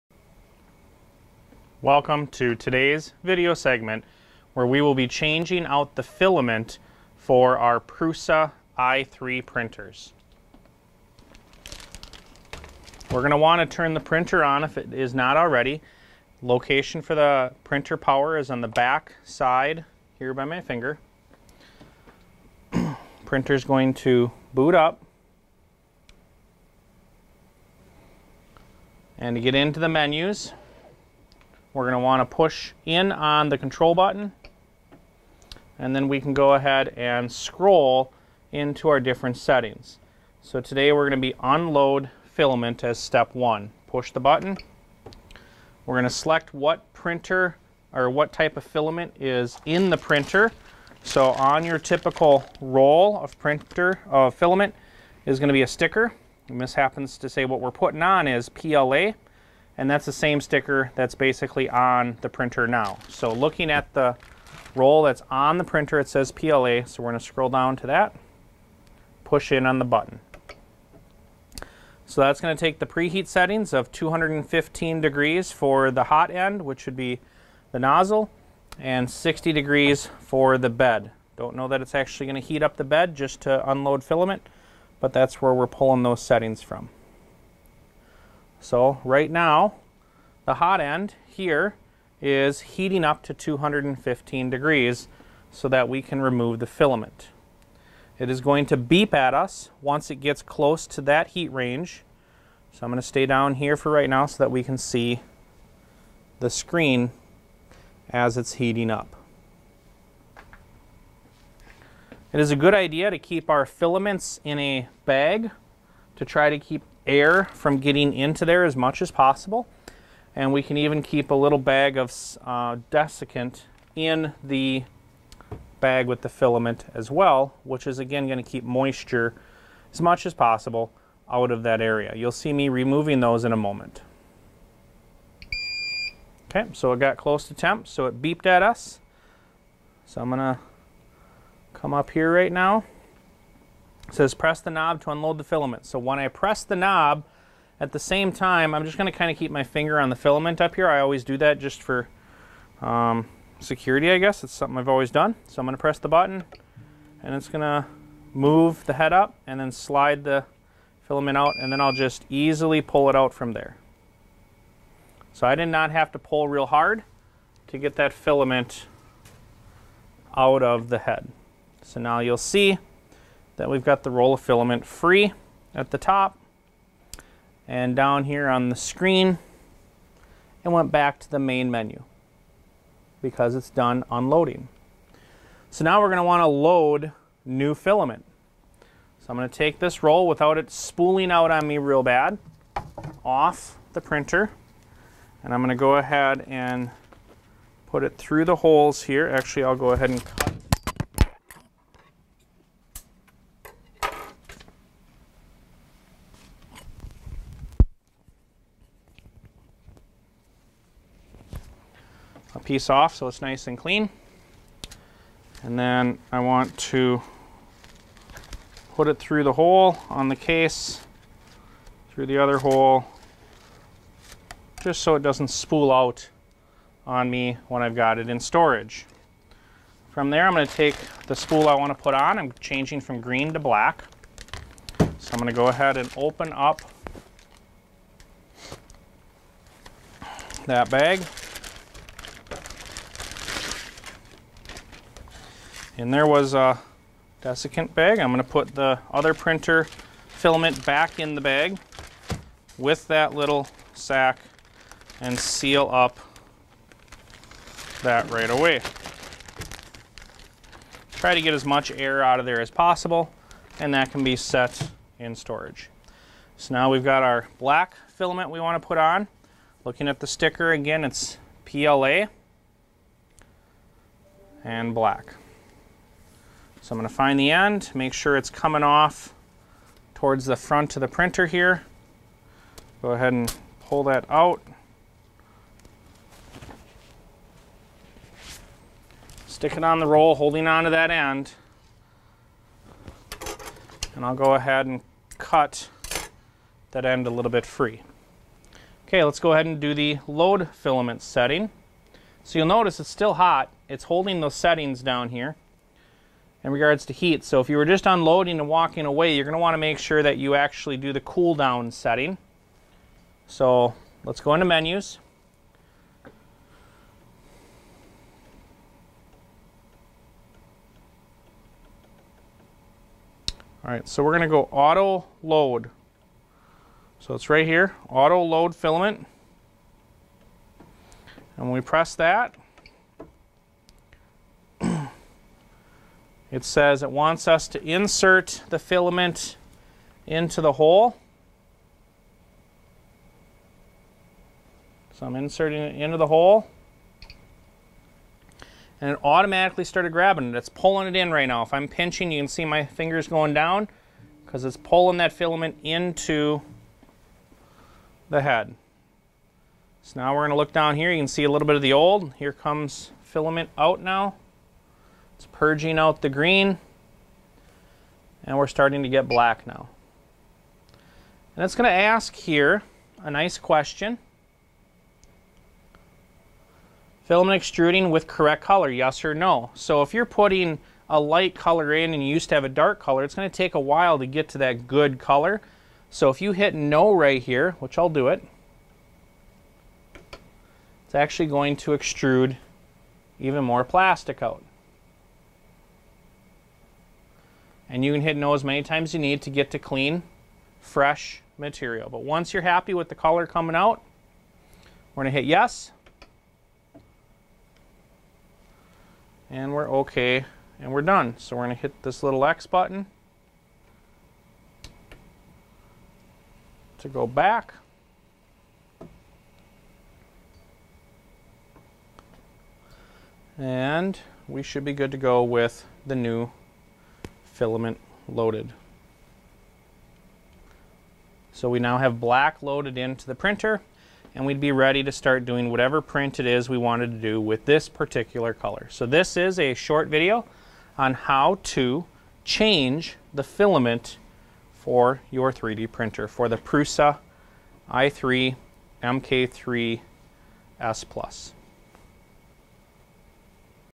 Prusa Printer: Changing Filament
In this short video, we walk "Step-by-Step" on how to change the Filament Roll on our Prusa Printers.